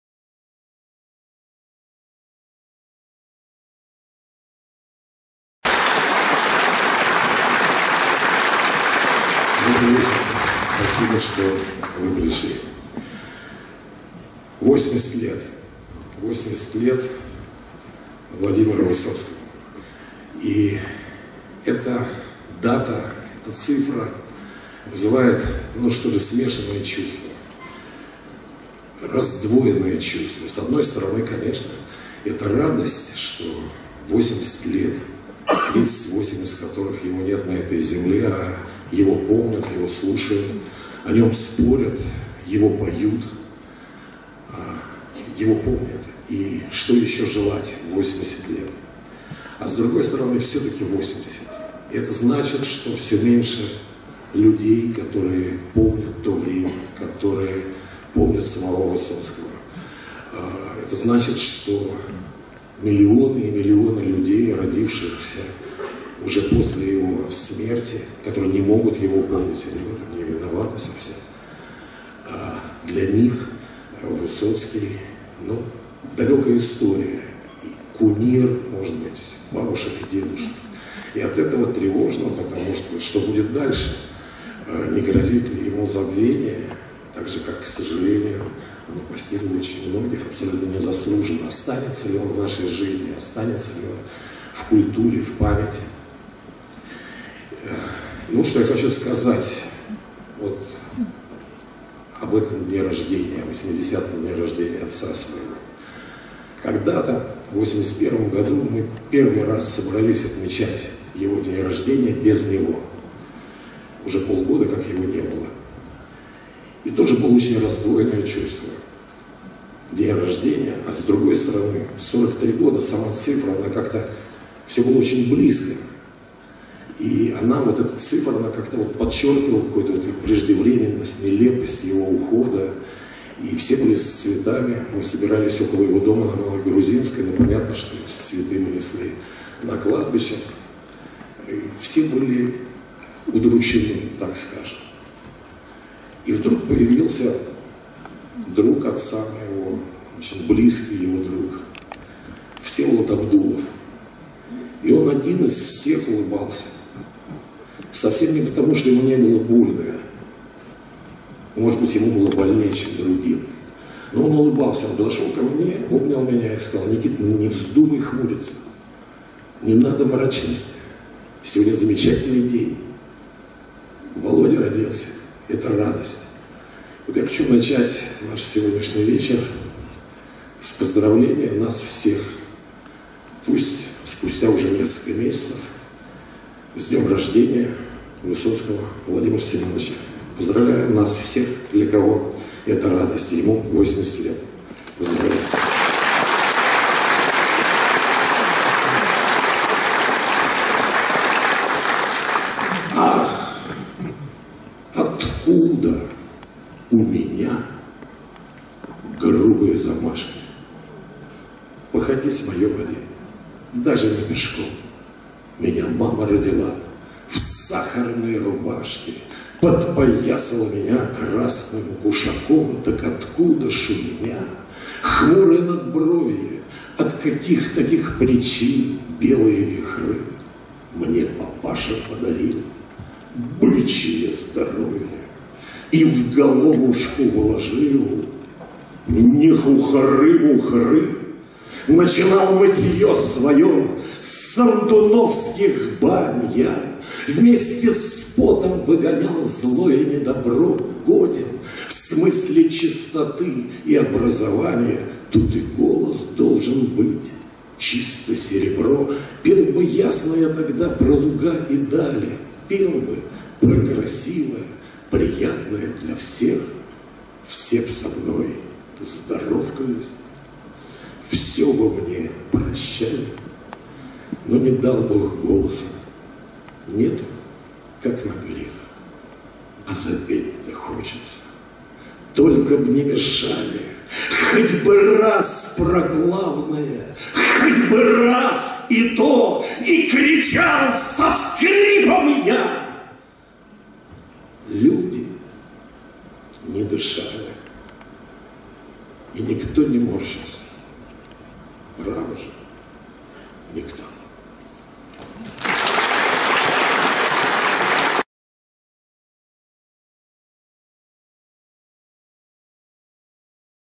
ДОРОГИ ВЫСОЦКОГО - (начало спектакля) - НИКИТА ВЫСОЦКИЙ - театр им. Моссовета, 26. 06. 18 г.
МУЗЫКАЛЬНО - ЛИТЕРАТУРНЫЙ СПЕКТАКЛЬ "ДОРОГИ ВЫСОЦКОГО." Автор Проекта - Александр Домогаров. Вступительное слово и стихотворение В. С. Высоцкого  "Ах, откуда у меня грубые замашки" в исполнении НИКИТЫ ВЫСОЦКОГО. Театр имени Моссовета, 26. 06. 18 г.